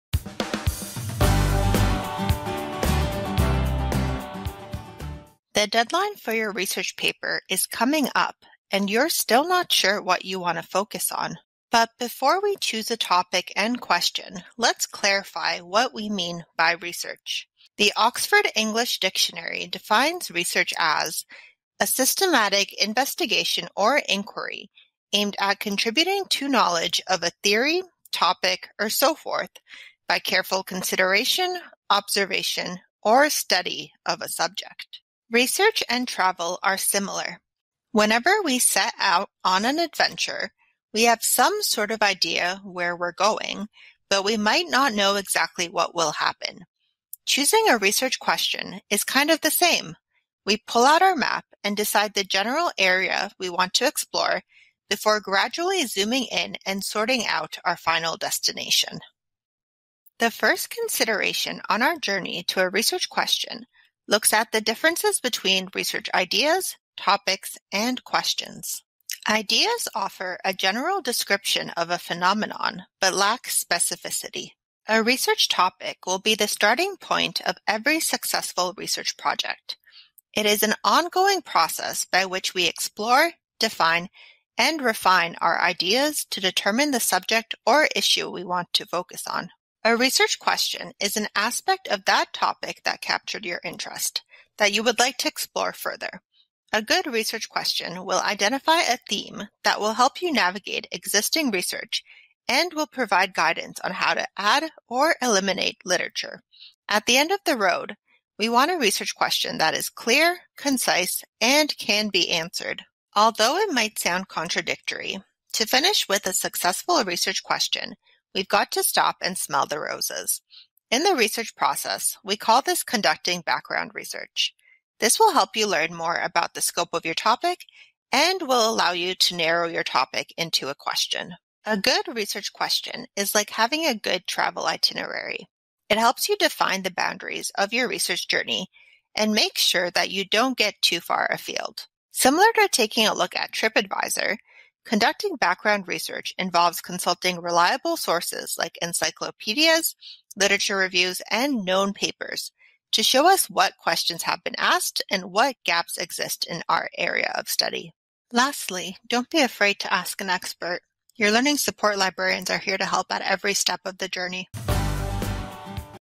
How Library Stuff Works - Choosing a Research Question
This video focuses on how to choose the best research question for your assignment.